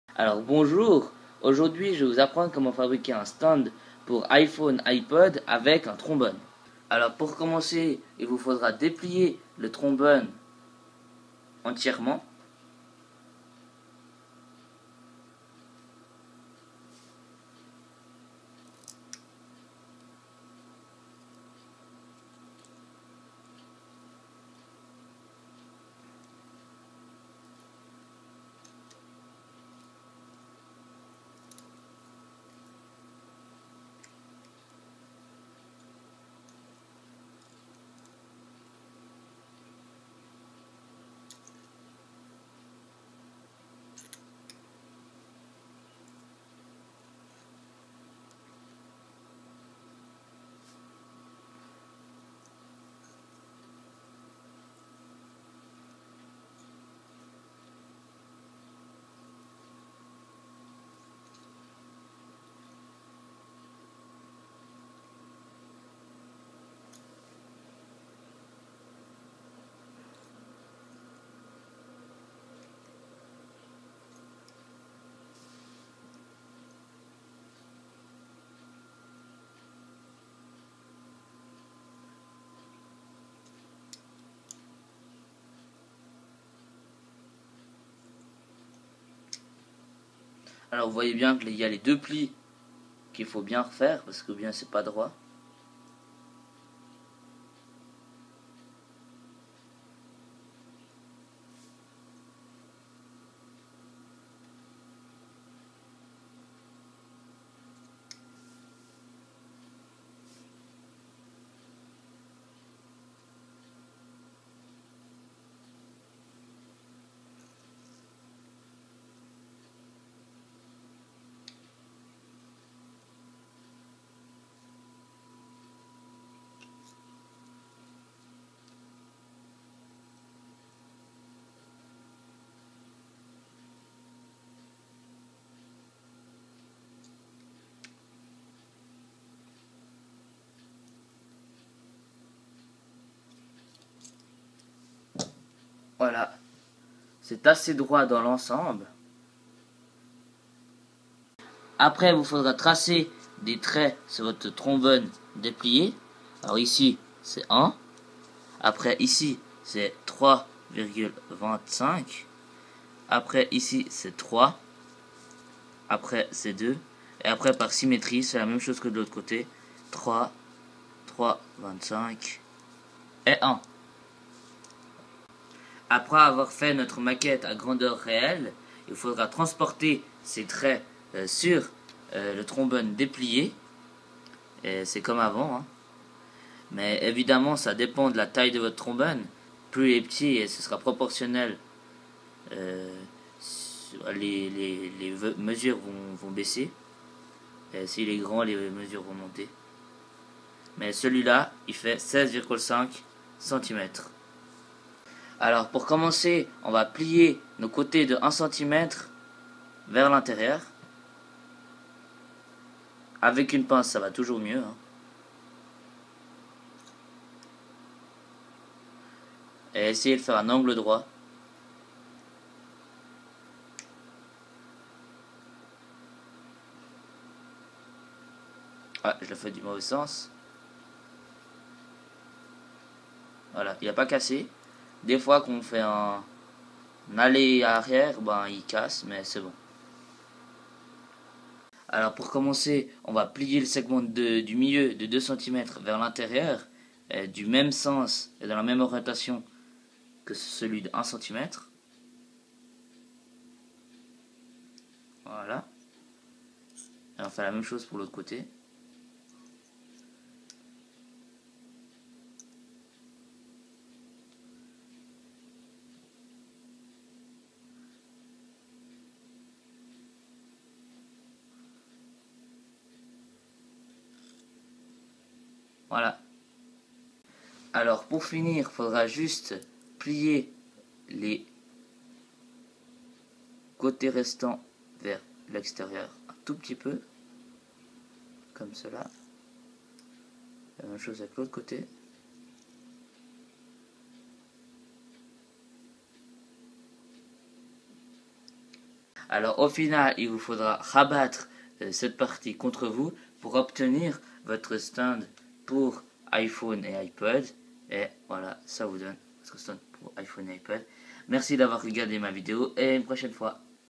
Faire un stand iPhone avec un trombone / Fabriquer un support pour son téléphone
Faire un stand iPhone avec un trombone. Vous avez besoin d'un trombone, grand de préférence, et d'une pince afin de le plier facilement. Le résultat obtenu vous permettra de laisser reposer votre smartphone sur un support, ce qui peut être pratique afin de regarder des vidéos avec des amis par exemple.

Si vous aussi désirez en faire et gagner ainsi de l'argent, n'hésitez pas à nous rejoindre ou bien regardez nos autres vidéos tutoriels